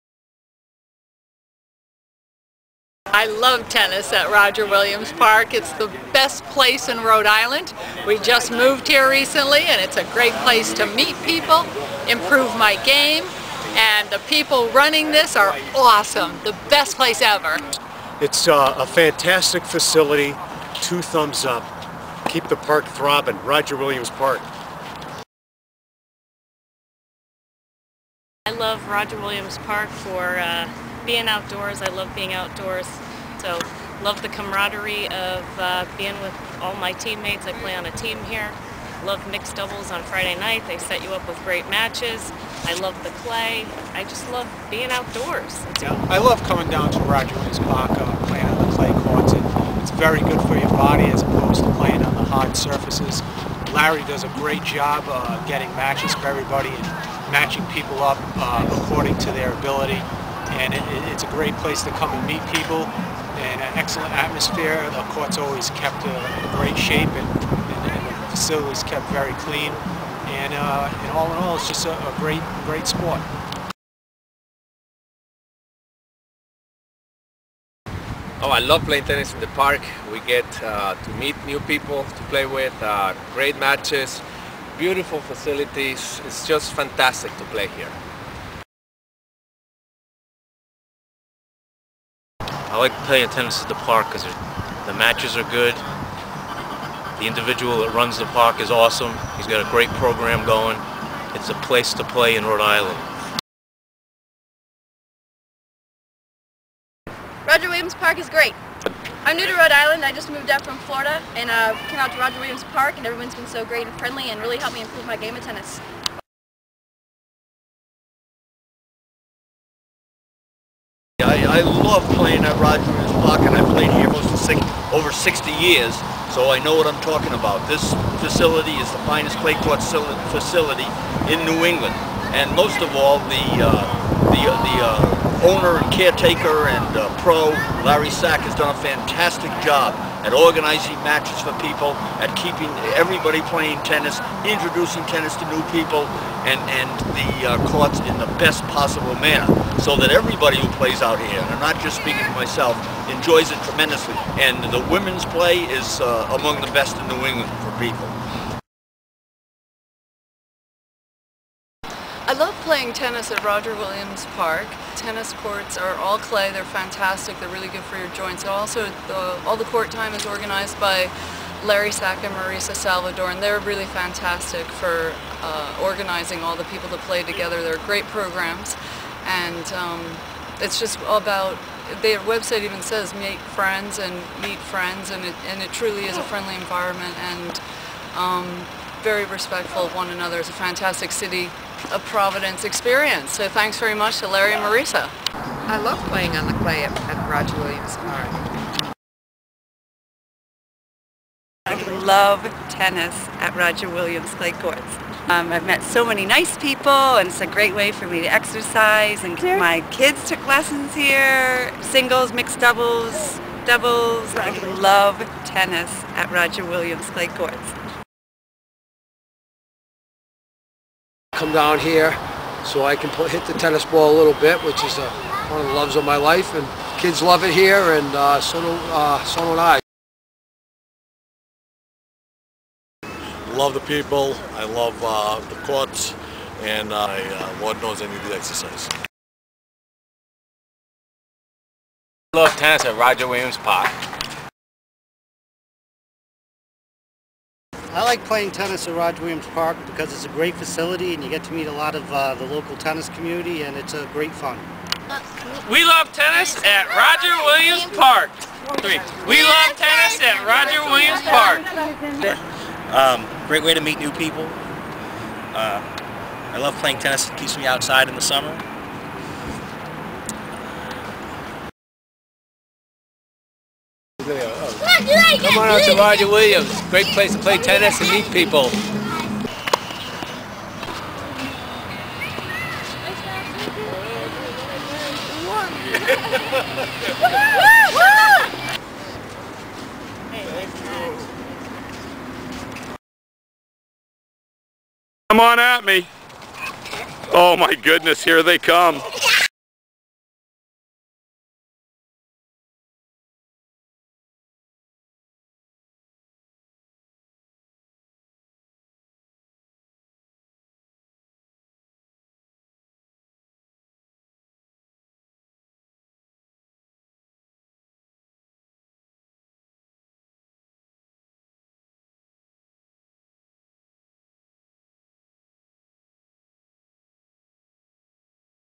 spot promo 2009
park friends promote Todd Morsilli Clay Court Tennis Center, Roger Williams Park